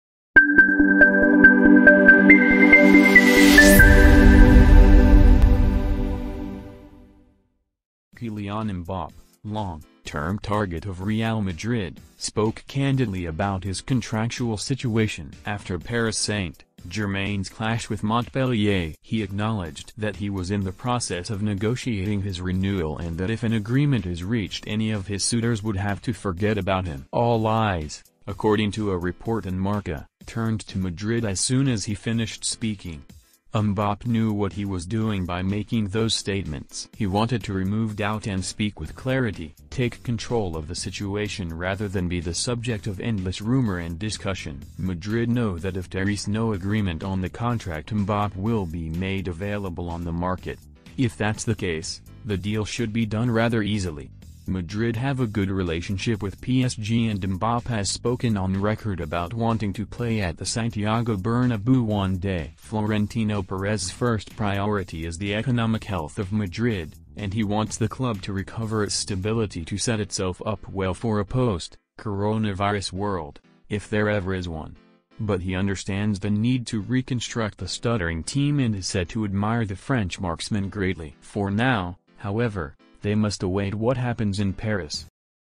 Kylian Mbappe puts the ball in Paris Saint-Germain’s court with Real Madrid waiting in the wing...
Kylian Mbappe puts the ball in Paris Saint-Germain’s court with Real Madrid waiting in the wings - news today

Kylian Mbappe, long-term target of Real Madrid, spoke candidly about his contractual situation after Paris Saint-Germain’s clash with Montpellier. He acknowledged that he was in the process of negotiating his renewal and that if an agreement is reached any of his suitors would have to forget about him. All eyes, according to a report ...